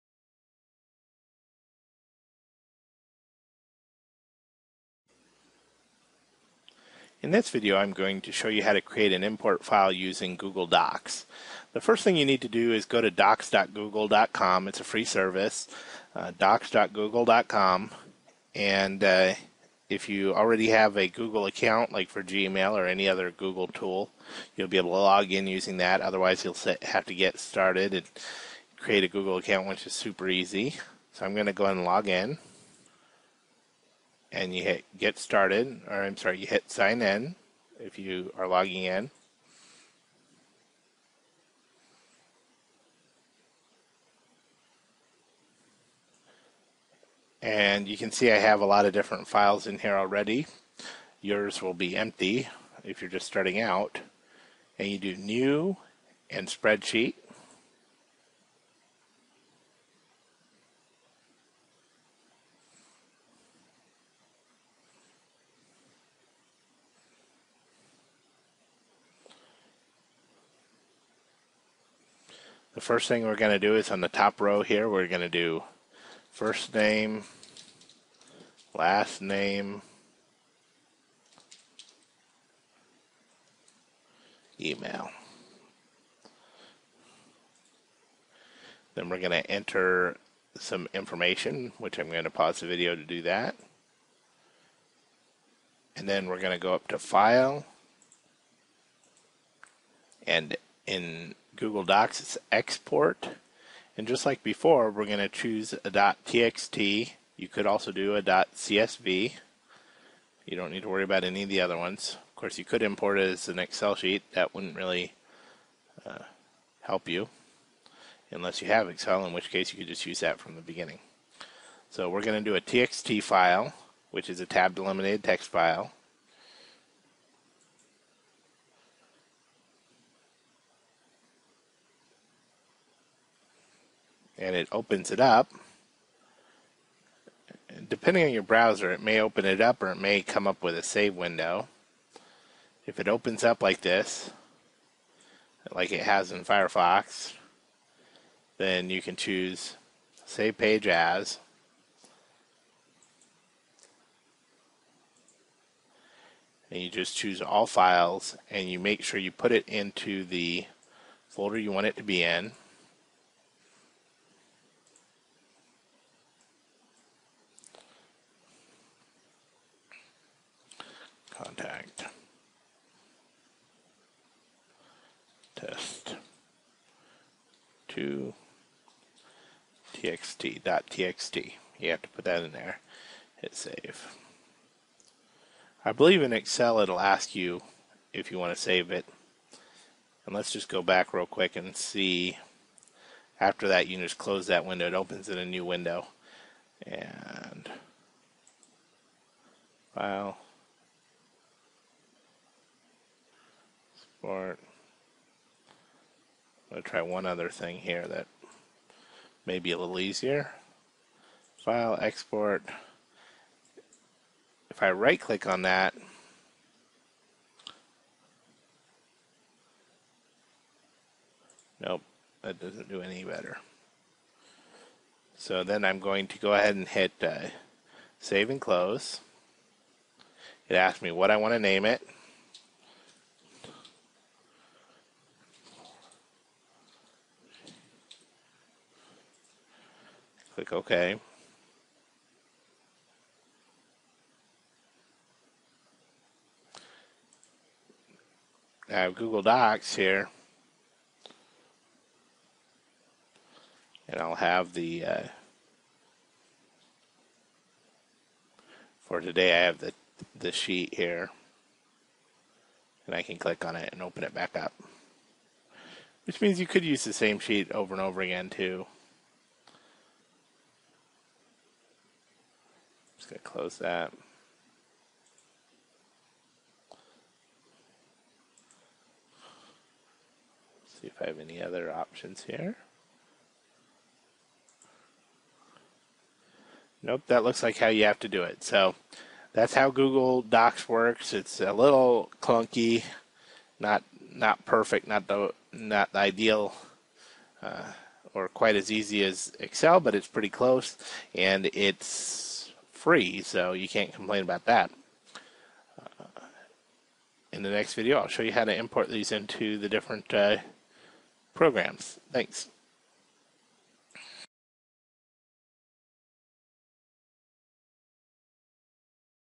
Import Contacts Into Your Social Network - Part 1b
- Social Networking Professor Free Video Training.

In this set of videos I show you how to import contacts into your social networks.

These videos are made up of the following:

1) How to create a CSV file using Excel,
1b) How to create a CSV using google docs
2) Import contacts into Facebook
3) Import contacts into Linkedin